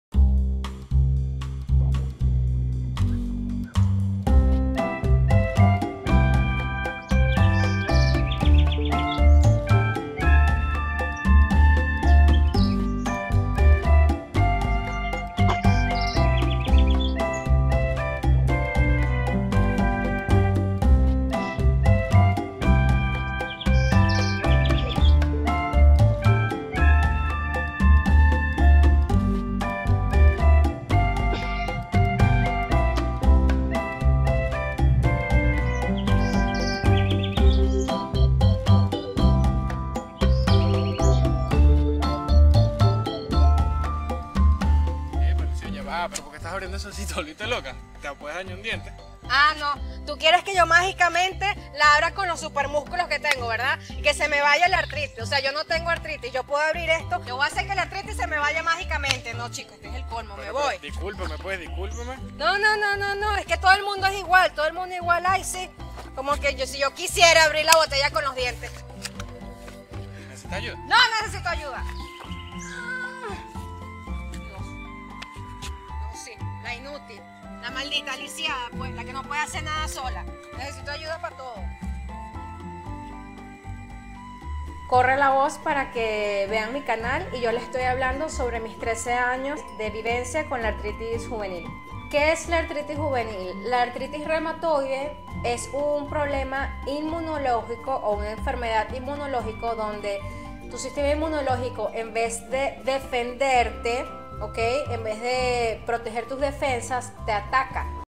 Vivir con Artritis: "La botella de agua"
Vivir con Artritis | Sketch: "La botella de agua" | Idea original de Lucía León. Valencia, Venezuela. 2016. ¡Deja tu comentario y corre la voz!

Éste video fue hecho con mucho cariño para ustedes, la idea es mostrar de una manera "jocosa" la cruda y triste realidad de lo que es realizar una tarea tan básica y sencilla, como es en este caso abrir una botella de agua sin tener quien te ayude. Para alguien normal esto puede ser absurdo, pero para alguien con artritis, como lo pueden ver, es complicado, frustrante , doloroso, y simplemente hay que ingeniárselas para poder realizar estas tareas del día a día. ¡Vamos a crear consciencia y a apoyarnos! PD: ¡Eternamente agradecida con Francisco, por tu amistad, tu cariño y tu compromiso para conmigo en éste proyecto! ♥

Editado y producido por Francisco Balza.